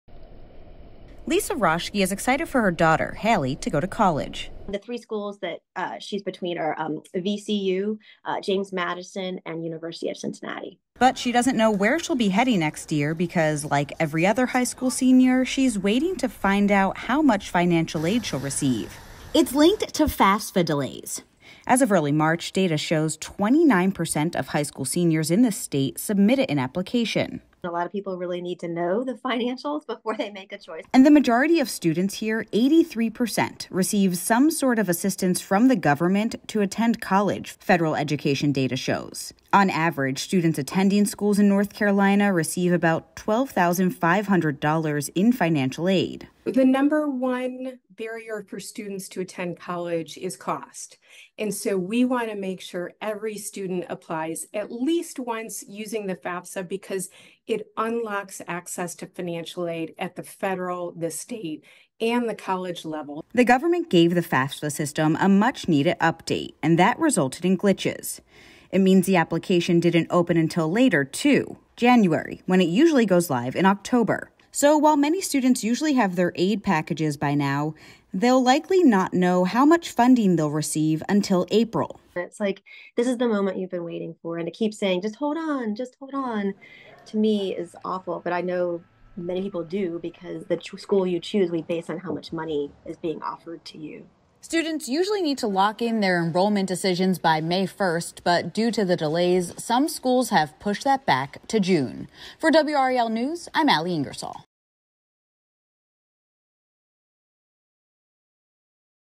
FAFSA delays put financial aid information, college decisions in limbo
About eight out of every 10 students in North Carolina receive some sort of federal financial assistance.